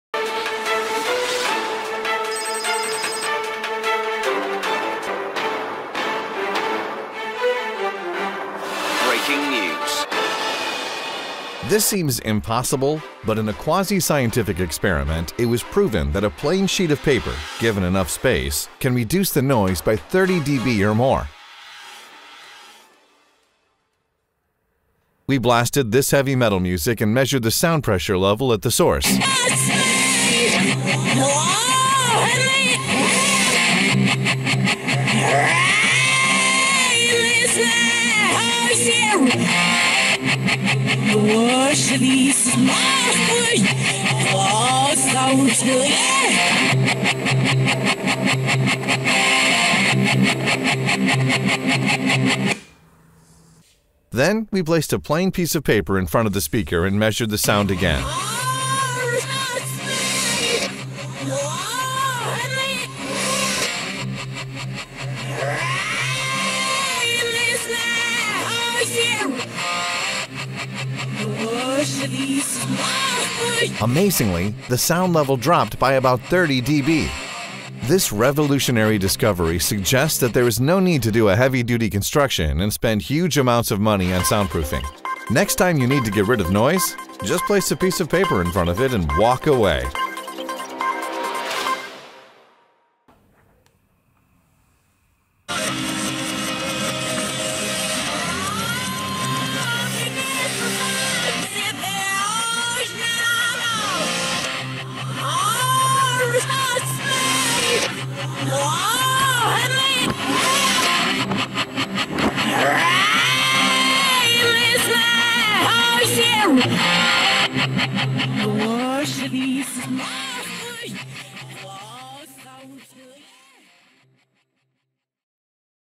Soundproofing Paper. Cheapest noise reduction method ever!
FULL DISCLOSURE of FACTS:
It is all real. All measurements are accurate within the margin of error of the instruments used.
This quasi-scientific experiment clearly shows that a plain sheet of paper, given enough space, can reduce the noise by 30dB or more!
We blasted this heavy metal noise and measured the Sound pressure level at the source.  (original sound track was scrambled to create noise source.)
Then we placed a plain piece of paper in front of the speaker and measured the sound again.
Amazingly, the sound level dropped by about 30 dB!
This revolutionary discovery suggests that there is no need to do a heavy duty construction and spend huge amounts of money on soundproofing. Next time you need to get rid of noise – just place a piece of paper in front of it and WALK AWAY!